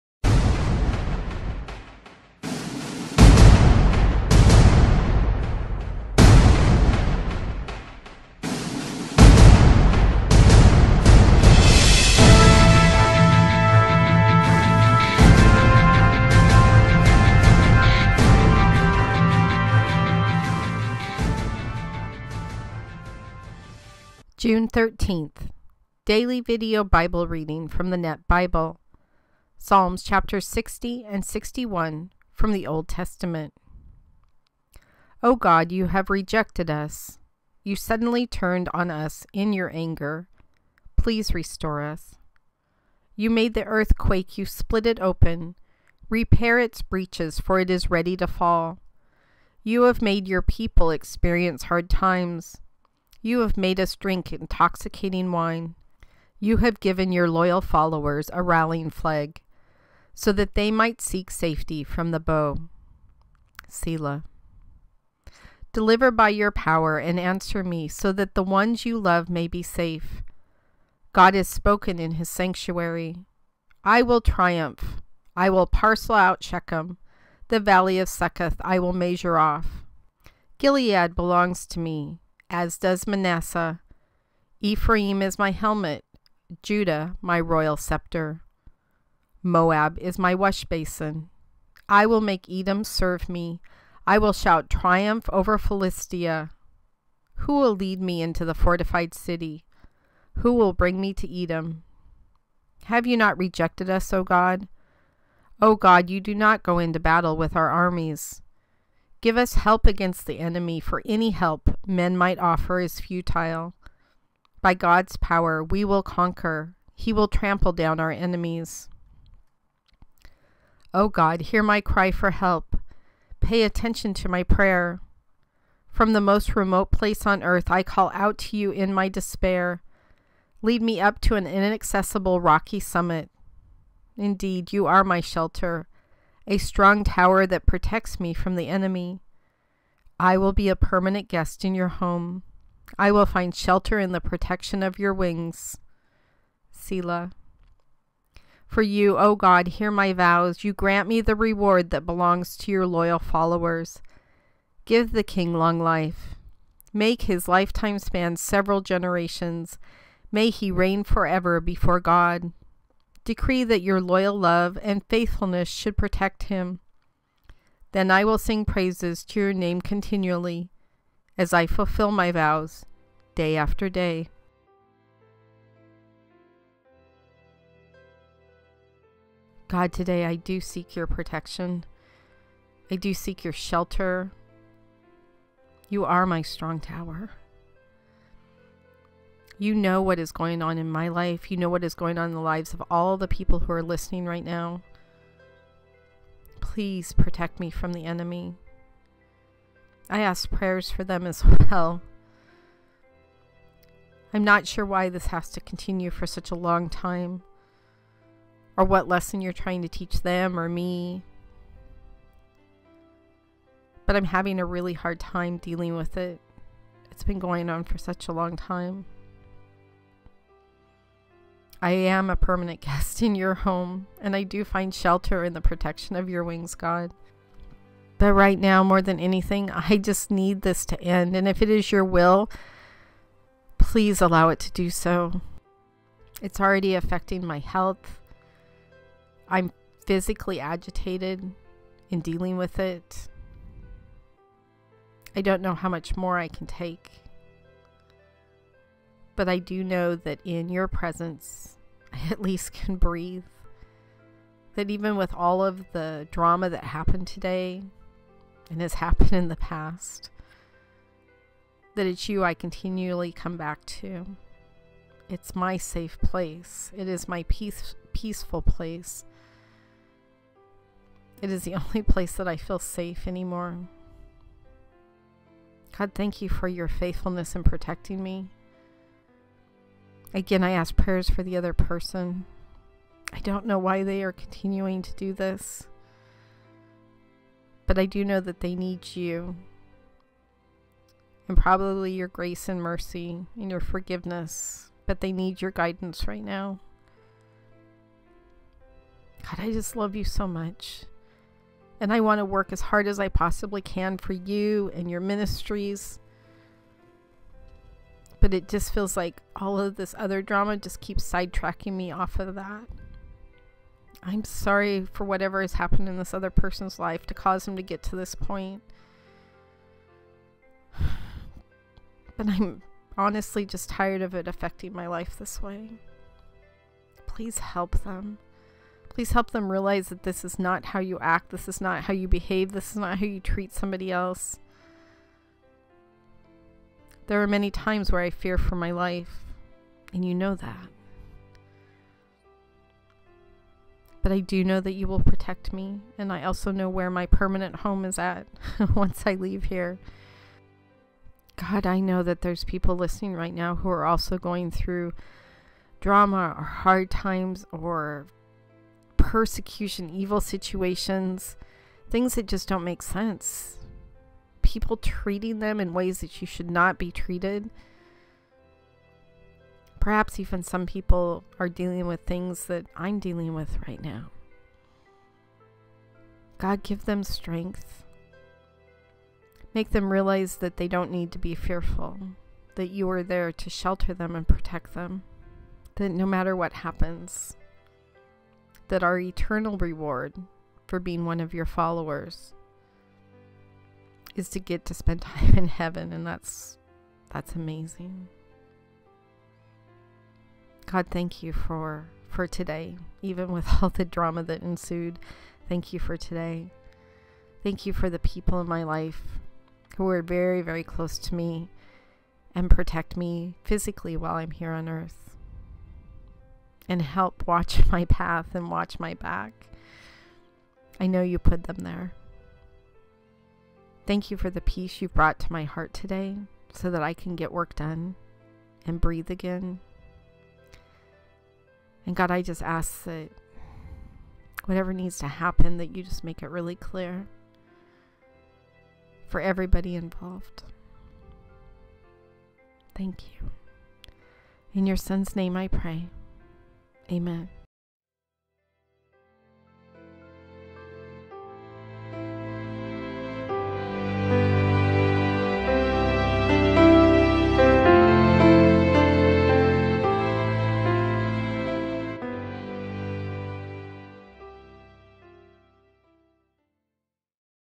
June 13 Psalms 60 and 61 Old Testament for the Daily Video Bible DVB
Psalms 60:1-12 and 61:1-8 - Psalm 60
For the music director; according to the shushan-eduth style; a prayer of David written to instruct others. It was written when he fought against Aram Naharaim and Aram-Zobah. That was when Joab turned back and struck down 12,000 Edomites in the Valley of Salt.
60:1 O God, you have rejected us.
You suddenly turned on us in your anger.
Please restore us!
60:2 You made the earth quake; you split it open.
Repair its breaches, for it is ready to fall.
60:3 You have made your people experience hard times;
you have made us drink intoxicating wine.
60:4 You have given your loyal followers a rallying flag,
so that they might seek safety from the bow. (Selah)
60:5 Deliver by your power and answer me,
so that the ones you love may be safe.
60:6 God has spoken in his sanctuary:
"I will triumph! I will parcel out Shechem;
the Valley of Succoth I will measure off.
60:7 Gilead belongs to me,
as does Manasseh!
Ephraim is my helmet,
Judah my royal scepter.
60:8 Moab is my washbasin.
I will make Edom serve me.
I will shout in triumph over Philistia."
60:9 Who will lead me into the fortified city?
Who will bring me to Edom?
60:10 Have you not rejected us, O God?
O God, you do not go into battle with our armies.
60:11 Give us help against the enemy,
for any help men might offer is futile.
60:12 By God's power we will conquer;
he will trample down our enemies.
Psalm 61
For the music director; to be played on a stringed instrument; written by David.
61:1 O God, hear my cry for help!
Pay attention to my prayer!
61:2 From the most remote place on earth
I call out to you in my despair.
Lead me up to an inaccessible rocky summit!
61:3 Indeed, you are my shelter,
a strong tower that protects me from the enemy.
61:4 I will be a permanent guest in your home;
I will find shelter in the protection of your wings. (Selah)
61:5 For you, O God, hear my vows;
you grant me the reward that belongs to your loyal followers.
61:6 Give the king long life!
Make his lifetime span several generations!
61:7 May he reign forever before God!
Decree that your loyal love and faithfulness should protect him.
61:8 Then I will sing praises to your name continually,
as I fulfill my vows day after day.